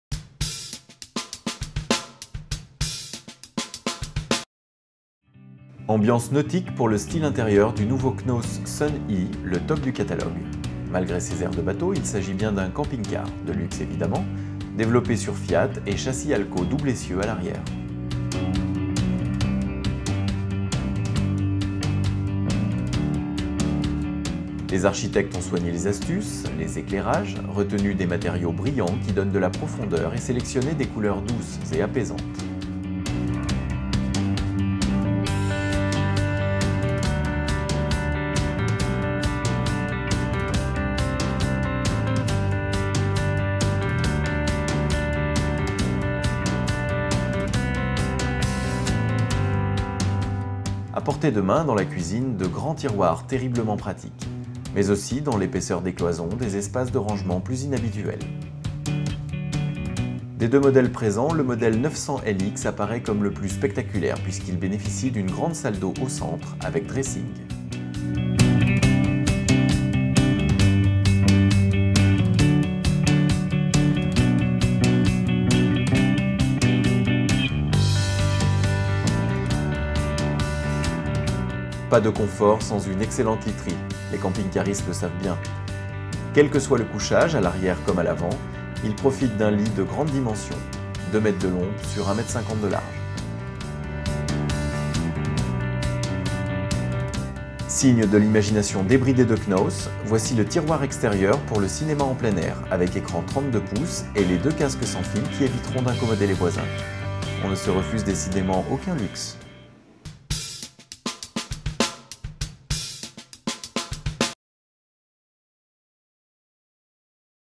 Knaus Sun I 900 LX : le camping-car poids lourd en version premium !
Knaus, le numéro un des ventes de camping-cars en Allemagne réintroduit à son catalogue une série de deux liners proposé sur châssis Fiat / Al-KO double essieux, les SUN-I 900 LX (lit central et salle de bain transversale) et SUN-I 900 LEG (lits séparés), avec à la clé un résultat particulièrement réussi en termes de design intérieur et extérieur, mais aussi sur le plan technologique.